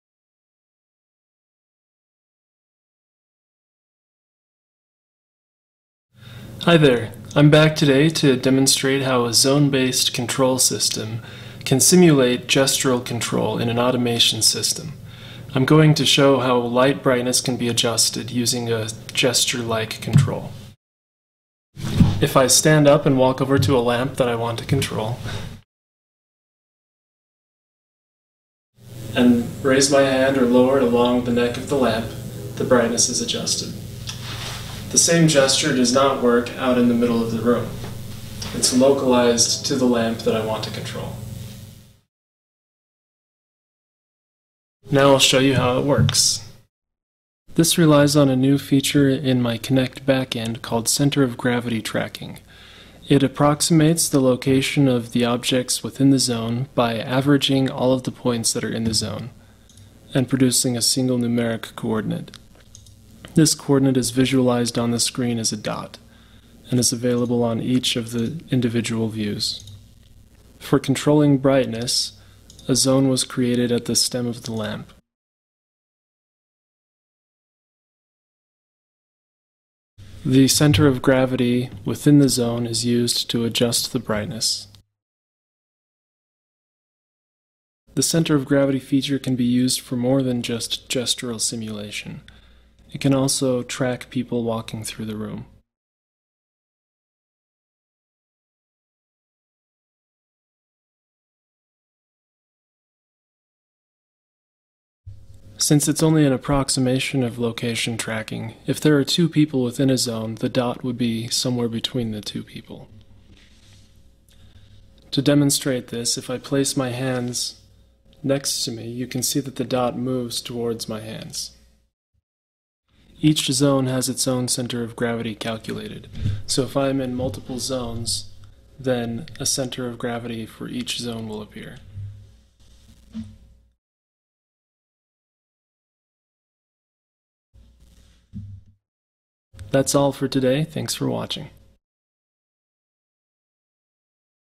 Gesture-like Light Control with Kinect
This video shows how zone-based control can be used to do gesture-like things on a low-CPU-power device.